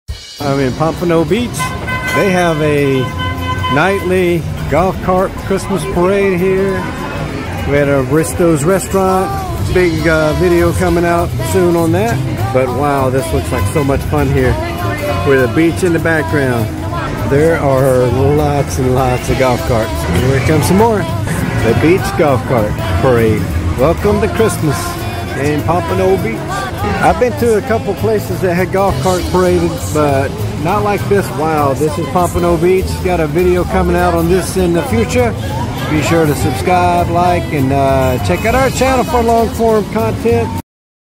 Experience the Magical Christmas Golf Cart Parade in Pompano Beach from Baresco's Restaurant
Pompano Beach Golf Cart Parade is on Check it out I've been to a couple places that had golf carts paraded but not like this one. I've already seen about 20 golf carts come through here. If you're anywhere near Fort Lauderdale Beach - North of Miami you gotta come check out the parade here Really cool. AI messed up the restaurant name. it's BARESCO!!!